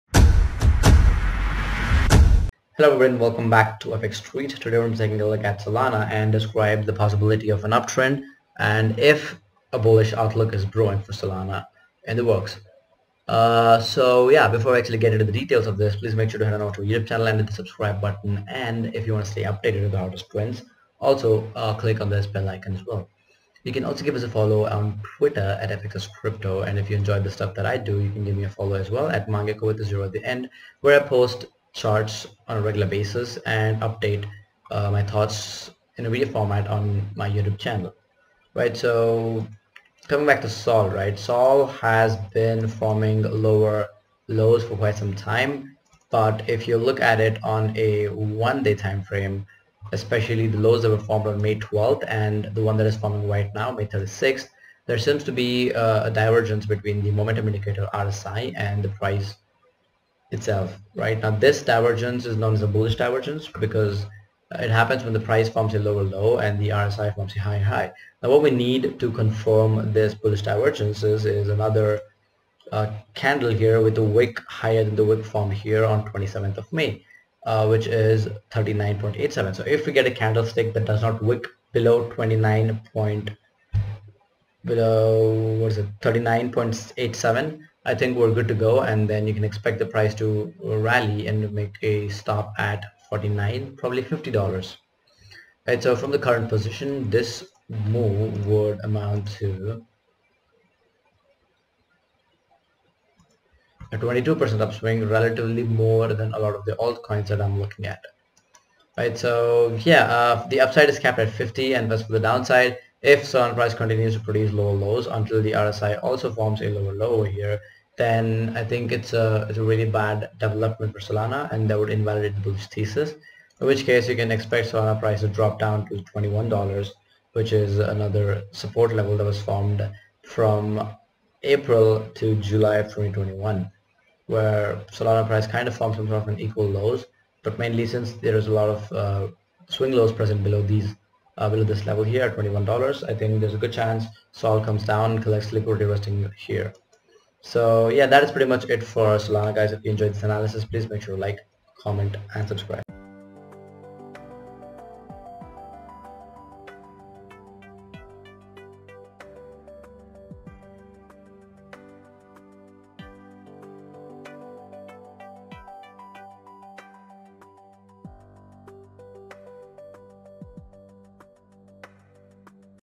Solana price can rebound for this good reason
A brief technical and on-chain analysis on Solana price. Here, FXStreet’s analysts evaluate where SOL could be heading next.